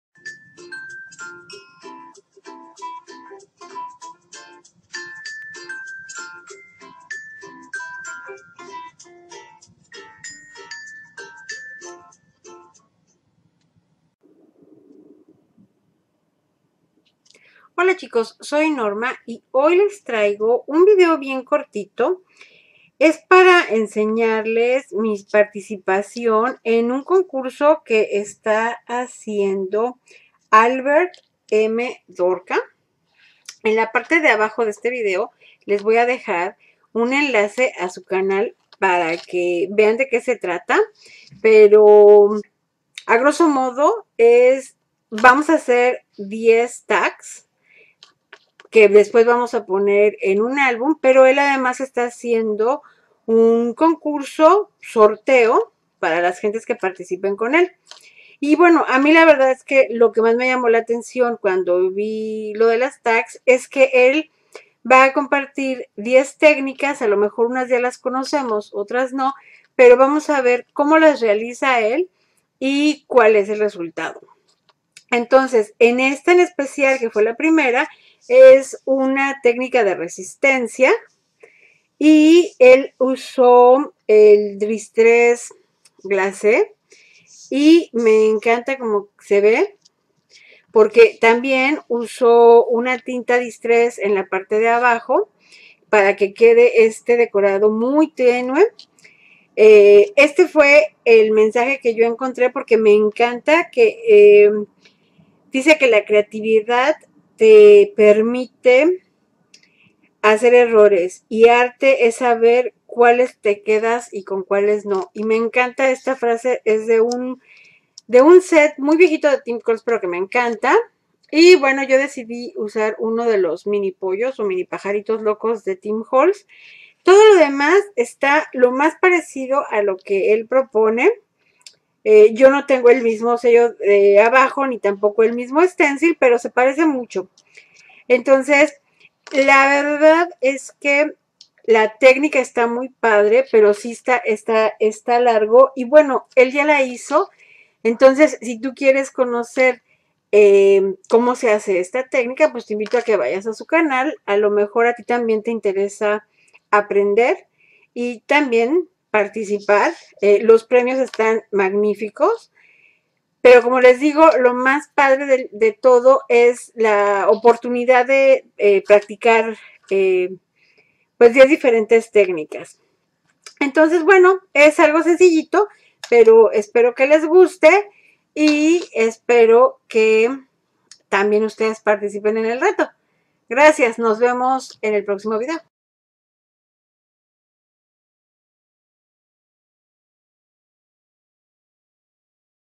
Tag 1 | Albert M Dorca INK | 10 sabados 10 tags | Distress Glaze
En este video te muestro mi participación en el taller de Albert M Dorca Ink, a quien puedes encontrar en: